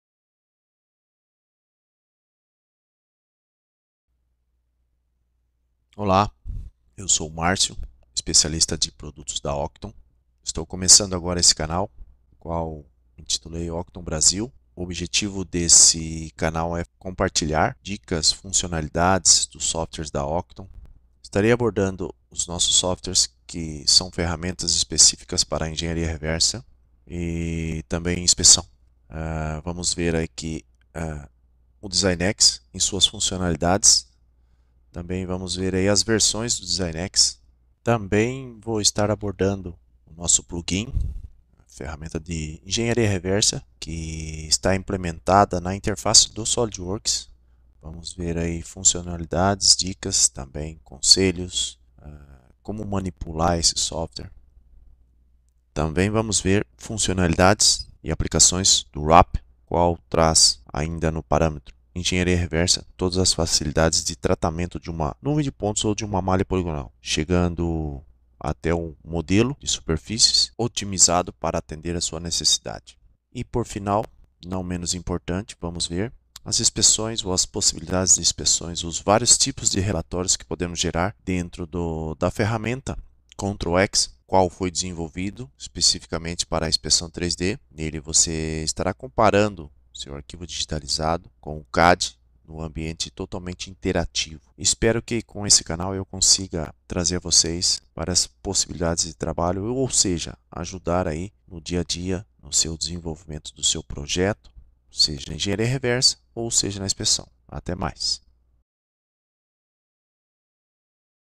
Descubra a Oqton, empresa de soluções de software de manufatura baseadas em IA
Este vídeo oferece uma breve introdução à Oqton e suas soluções para fabricação aditiva e digitalização 3D.